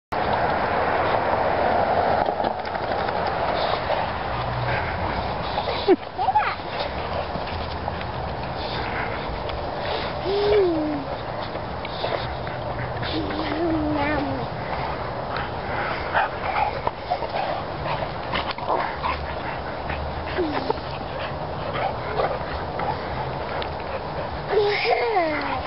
The Boxers Teach Elsie How To Drink From The Hose.
14 month old Elsie drinking from the hose in the yard with the dogs.